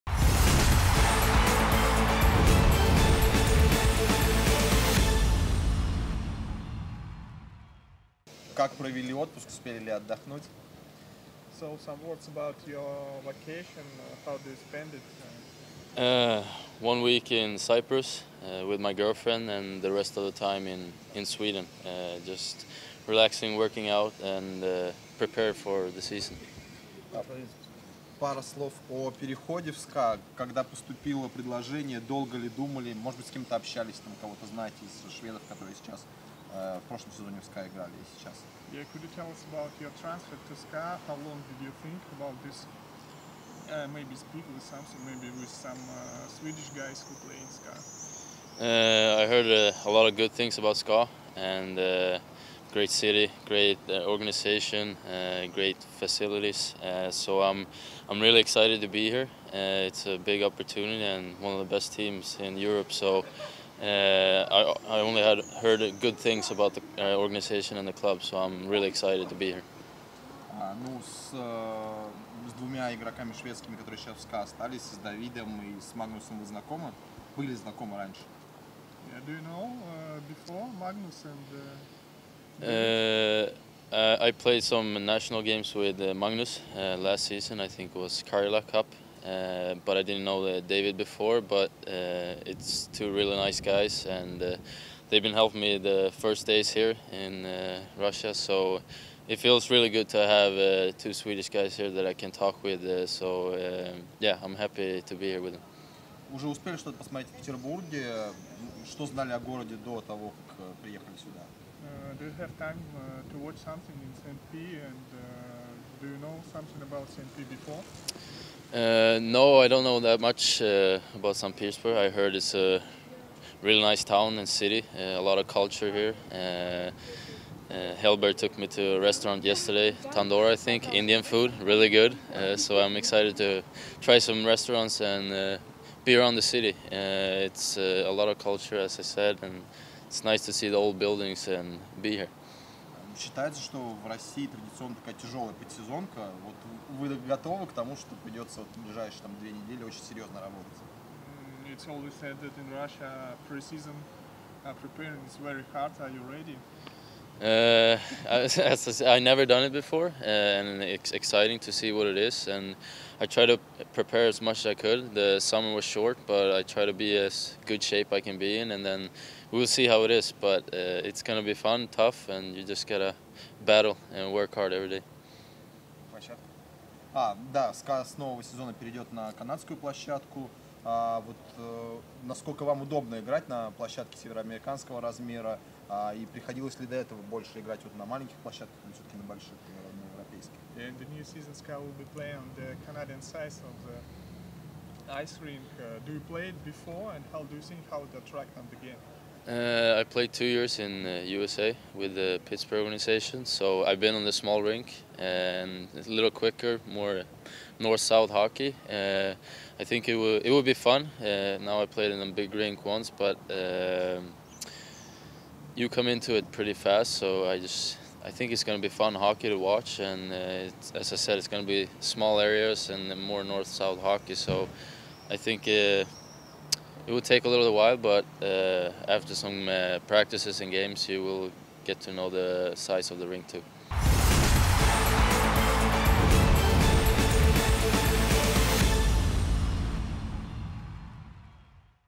​Лукас Бенгтссон: «Слышал про СКА много хорошего»
Вот, знакомьтесь - это наш новичок Лукас Бенгтссон. Сегодня третий день медосмотра, и защитник прошел его вместе с остальными легионерами. Узнаём, как у Лукаса дела после отпуска.